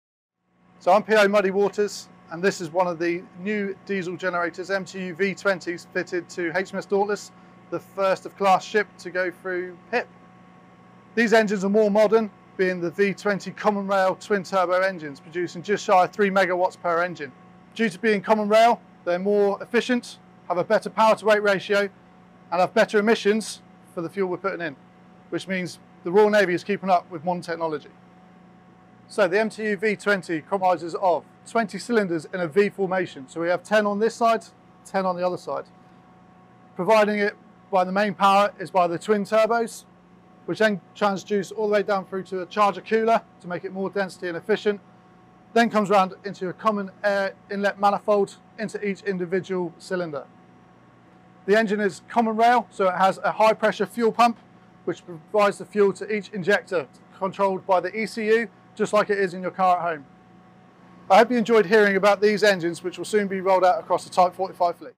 New diesel generators fitted to Type 45 destroyers
Video: Royal Navy